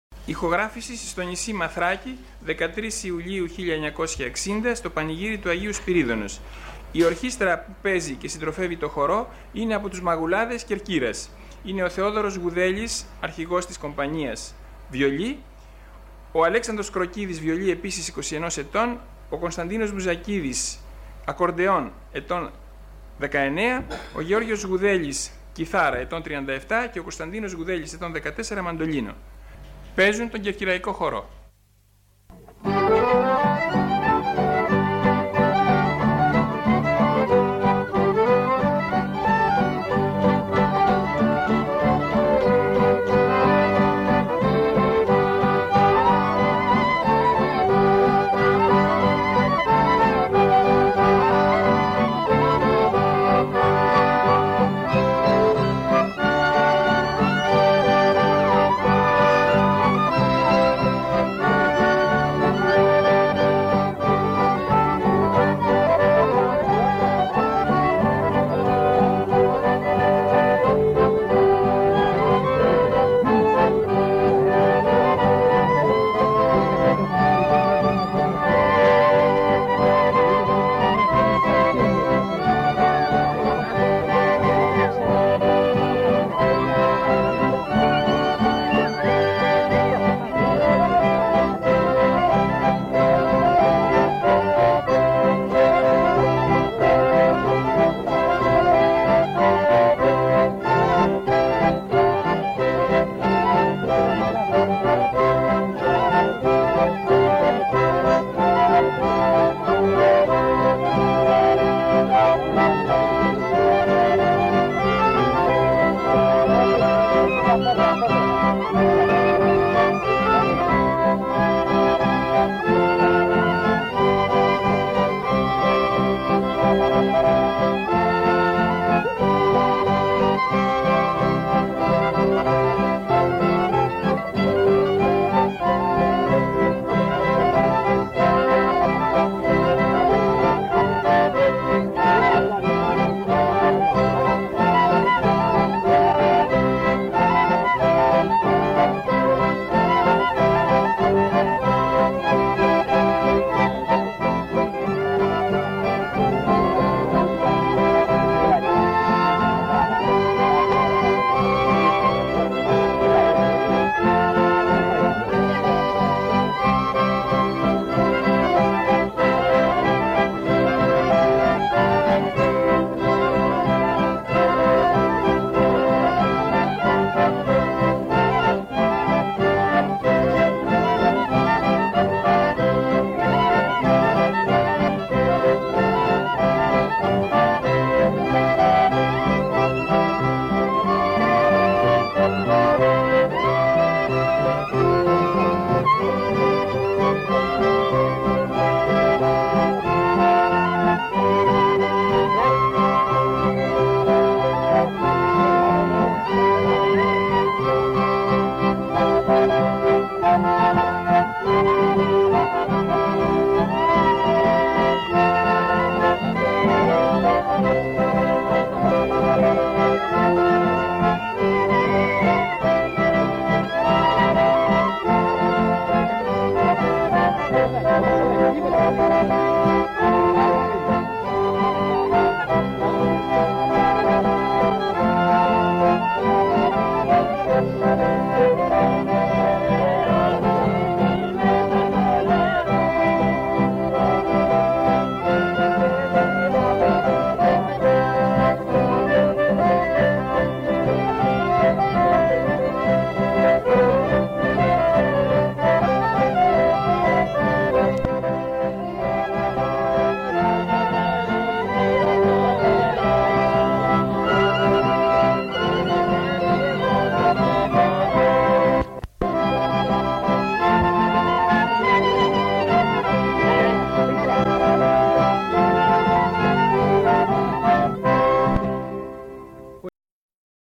Πανηγύρι στο Μαθράκι το 1960
Ένα σπάνιο ηχητικό ντοκουμέντο από το πανηγύρι του Αγ. Σπυρίδωνος στο Μαθράκι το ετος 1960!!..